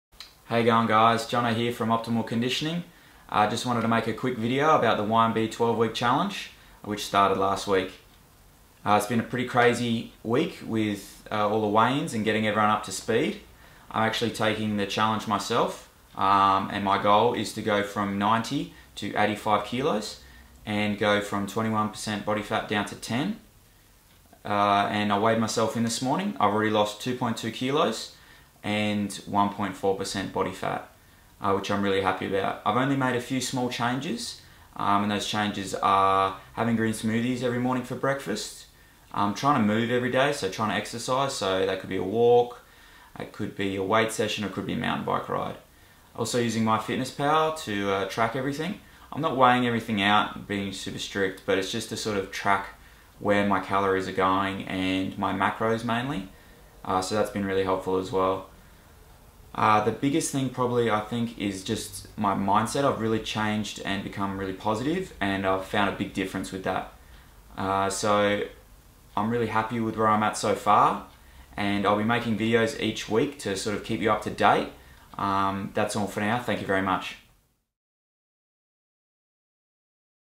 YNB 12 Week Challenge week 1
The YNB 12 week challenge is a Genesis Fitness program run to help people re-find their fitness.
Follow me as I take pat in the challenge not only as a Trainer but a participant.

Week 1